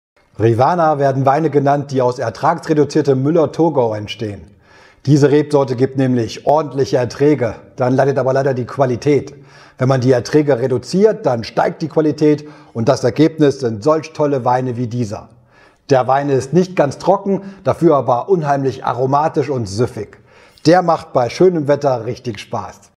Weingut Kiefer Rivaner
Der Rivaner vom Weingut Kiefer ist ein Weisswein aus Deutschland, genauer gesagt aus Baden.